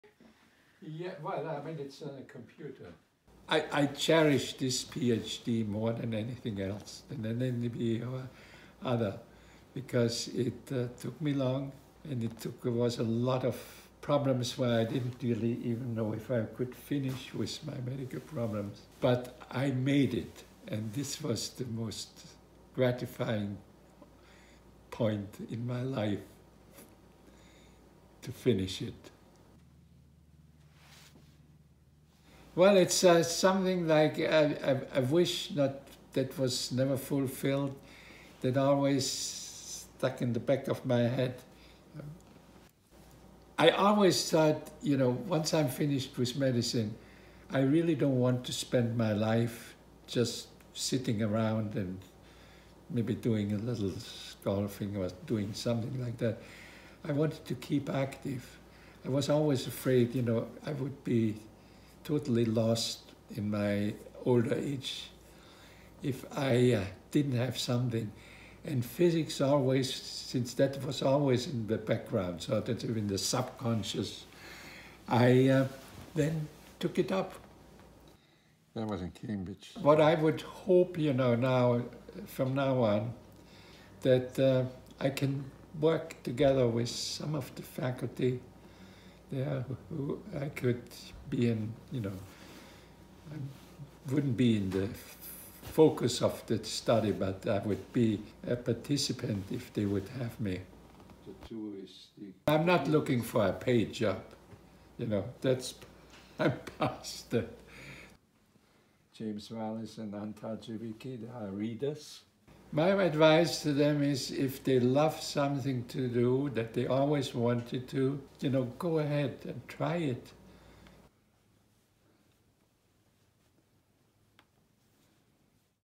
Man fulfills physicist dream, earns Ph.D. at 89
An 89-year-old man in Rhode Island has earned his Ph.D. and became a physicist after working toward it for two decades and thinking about it for a lifetime. Manfred Steiner defended his dissertation at Brown University in Providence. (Nov. 11)